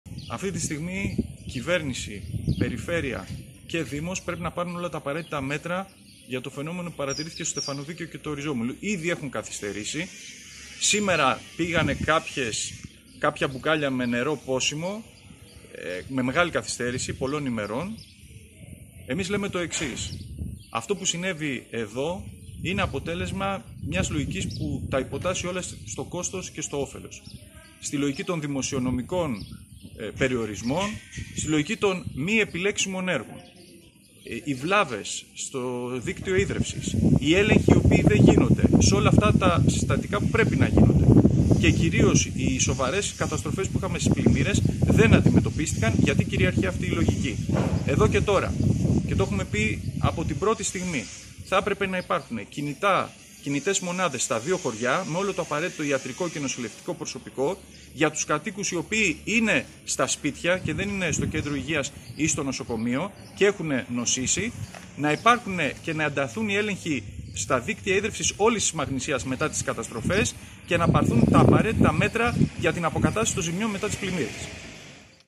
Β. Μεταξάς: Να ληφθούν μέτρα προστασίας για τα κρούσματα γαστρεντερίτιδας
Επικίνδυνες διαστάσεις έχει λάβει η υπόθεση με τα δεκάδες κρούσματα γαστρεντερίτιδας στα χωριά Ριζόμυλος και Στεφανοβίκειο Μαγνησίας, με τις μέχρι τώρα ενδείξεις να ενοχοποιούν το κοινό δίκτυο ύδρευσης.

Μέχρι στιγμής έχουν εντοπιστεί περισσότερα από 100 κρούσματα. Την ανησυχία των κατοίκων ενίσχυσε ο θάνατος μιας 90χρονης από τον Αλμυρό, η οποία νοσηλευόταν στο Νοσοκομείο Βόλου με συμπτώματα γαστρεντερίτιδας.

Τα σχολεία και τα νηπιαγωγεία έκλεισαν, οι μαθητές Γυμνασίου δίνουν εξετάσεις στο Γυμνάσιο του Βελεστίνου. Η υπόθεση έχει προκαλέσει εισαγγελική παρέμβαση.

Σύμφωνα με τον ΕΟΔΥ, με βάση τα πρώτα αποτελέσματα του εργαστηριακού ελέγχου των δειγμάτων νερού, «υπάρχει μικροβιολογική επιβάρυνση σε δείγματα από το περιφερειακό δίκτυο και τη δεξαμενή. Συγκεκριμένα, εκτός από μικροβιακούς δείκτες κοπρανώδους μόλυνσης που ανευρέθησαν σε πολλά σημεία δειγματοληψιών ανιχνεύθηκε επιπλέον Salmonella spp στη δεξαμενή και σε δύο επιπλέον δείγματα του περιφερειακού δικτύου. Ο εργαστηριακός έλεγχος των δειγμάτων νερού και η ταυτοποίηση των παθογόνων θα συνεχιστούν τις επόμενες ημέρες».

Όλες αυτές τις μέρες το δίκτυο ύδρευσης λειτουργούσε και μόλις χθες απαγορεύτηκε η οποιαδήποτε επαφή με το νερό. Τα δε πρώτα εμφιαλωμένα νερά έφτασαν την Κυριακή 2 Ιούνη! Μέχρι και σήμερα, στα δύο χωριά δεν υπάρχουν κινητές μονάδες για την παροχή ιατρικής φροντίδας σε ανθρώπους που έχουν νοσήσει και είναι στα σπίτια τους.

«Ήδη έχουν καθυστερήσει κυβέρνηση, Περιφέρεια και δήμος για την λήψη των αναγκαίων μέτρων στα χωριά όπου εμφανίστηκαν δεκάδες κρούσματα γαστρεντερίτιδας», τόνισε ο βουλευτής Μαγνησίας του ΚΚΕ Βασίλης Μεταξάς, που επισκέφτηκε χθες την περιοχή του δήμου Ρήγα Φεραίου.

Είχε συναντήσεις και ενημερώθηκε από την διευθύντρια του Κέντρου Υγείας Βελεστίνου Φανή Σαρηγιάννη, τον αντιπεριφερειάρχη Υγείας Ευάγγελο Αποστόλου και το κλιμάκιο του ΕΟΔΥ.

Σε δηλώσεις του τόνισε ότι έπρεπε από την πρώτη μέρα να δοθεί εμφιαλωμένο νερό, να υπάρχουν κινητές υγειονομικές μονάδες, να γίνουν έλεγχοι σε όλο το δίκτυο ύδρευσης της Μαγνησίας και να αντιμετωπιστούν οι ζημιές μετά τον «Ντάνιελ». Οπως χαρακτηριστικά είπε, «αποδεικνύεται για άλλη μια φορά πως η διαφύλαξη της υγείας των λαϊκών οικογενειών δεν συμβαδίζει με τη λογική του κόστους - οφέλους, της δημοσιονομικής πειθαρχίας, των "επιλέξιμων έργων", που αποτελούν διαχρονικές δικαιολογίες όλων των κυβερνήσεων - και των κομμάτων τους στην Τοπική Διοίκηση - ώστε να μην παίρνουν τα απαραίτητα μέτρα για την προστασία των υδάτων και του λαού που τα καταναλώνει. Αντιθέτως, όλοι τους διαδοχικά προωθούν την ιδιωτικοποίηση και εμπορευματοποίηση του νερού, και σ' αυτόν τον στόχο θέτουν τις προτεραιότητές τους».

Οι κάτοικοι ζητούν επίσημη ενημέρωση για το τι πρέπει να κάνουν και που να απευθύνονται. Να στηθούν κινητά ιατρεία ζήτησε ο Αποστόλης Παπαϊωάννου, επικεφαλής της «Λαϊκής Συσπείρωσης» Δήμου Ρήγα Φεραίου.